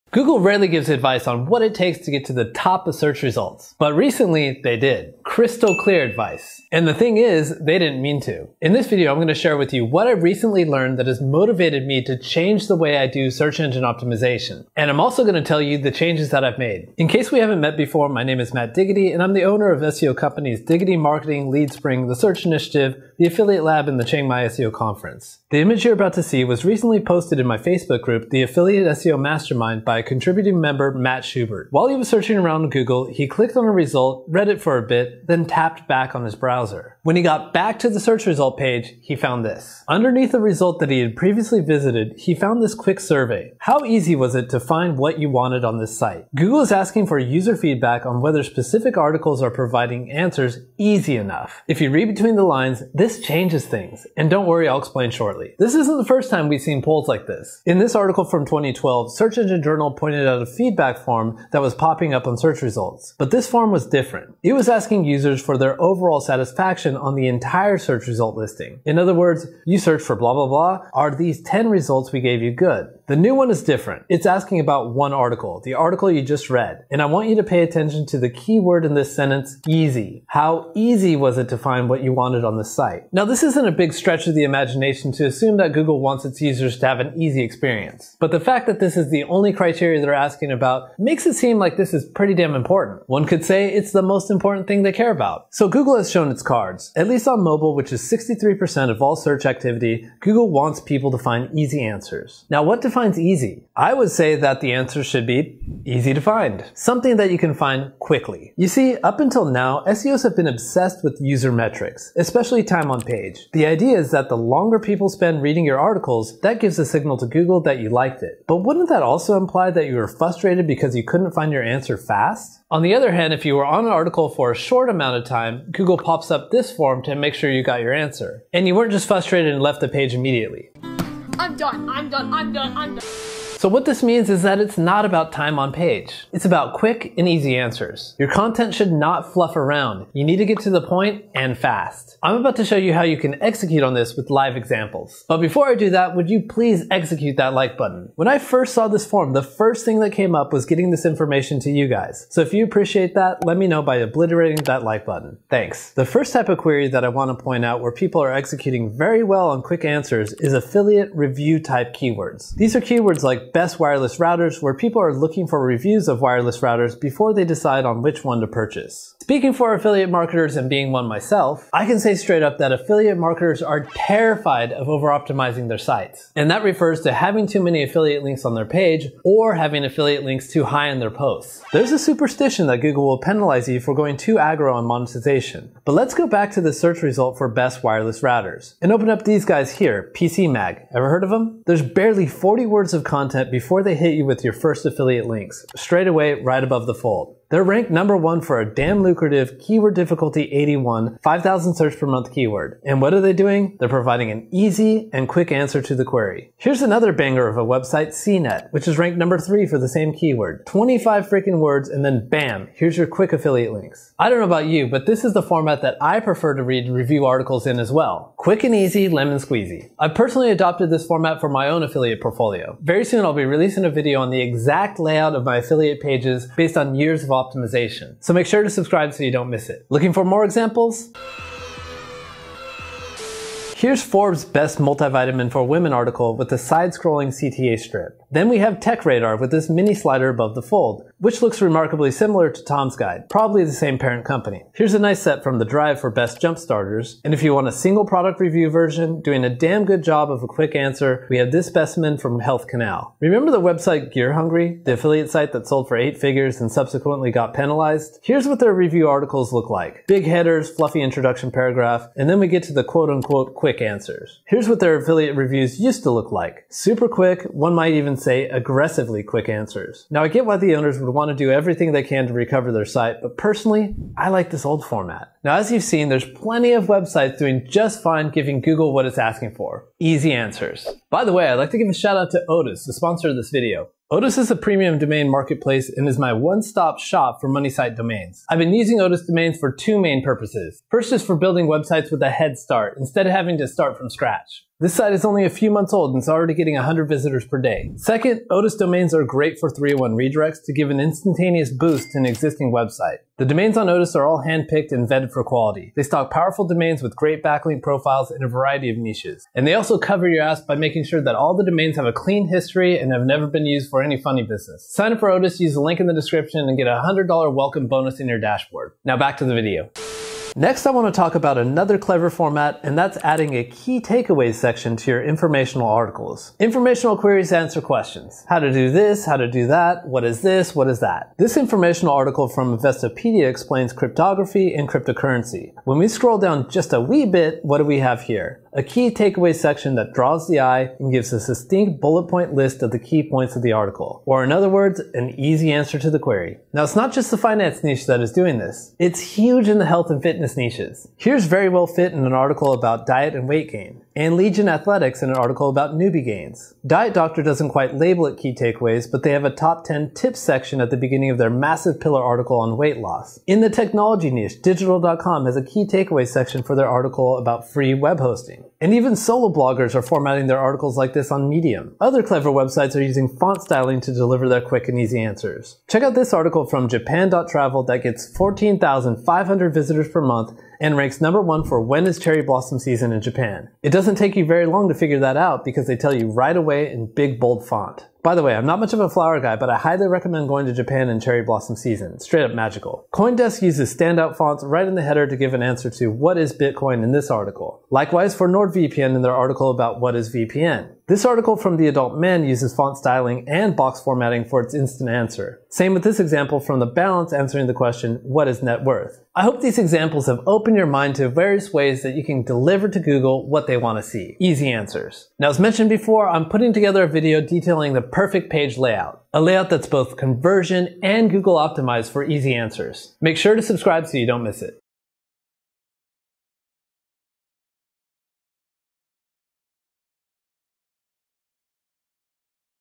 Google Just Messed Up! We Now Know EXACTLY What They Want
Now we know what Google wants. Find out what I’ve recently learned that has changed the way I do search engine optimization and what you should do TODAY ...

Here's what's covered:
00:00 Intro
00:25 What We Found
02:37 Affiliate Keywords
05:23 Key Takeaways
06:16 Font Styling